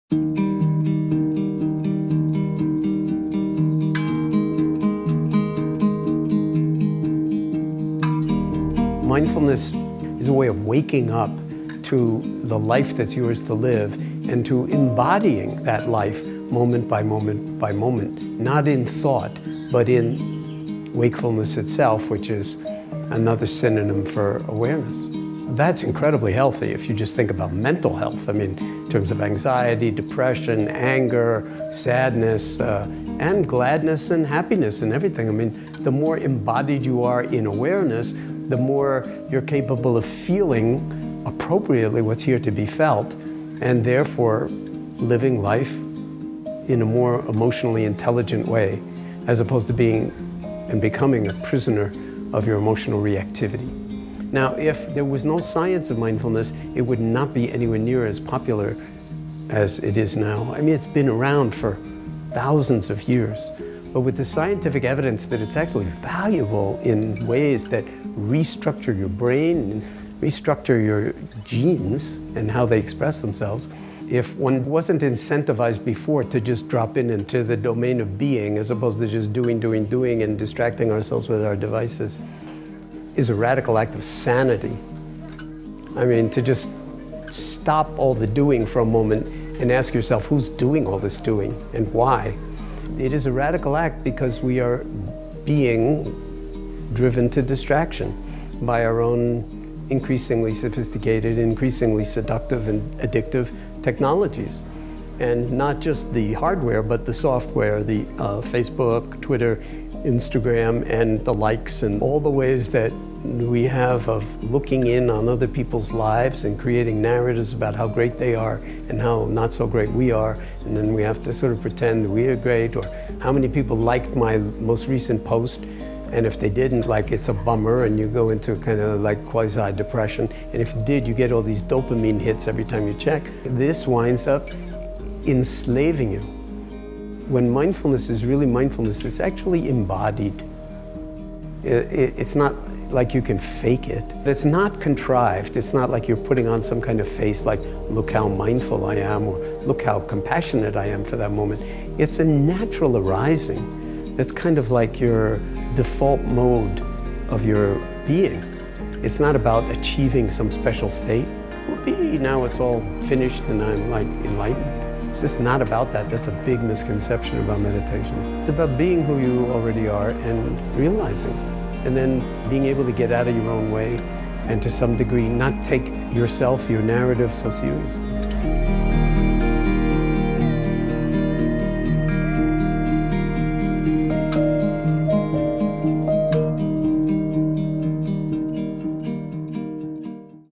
Behind the Scenes With Jon Kabat-Zinn
Go behind the scenes at Experience Life's September 2018 cover shoot with renowned mindfulness teacher Jon Kabat-Zinn to learn more about the effects of mindfulness on your health. Read the full story at ELmag.com/jonkabatzinn.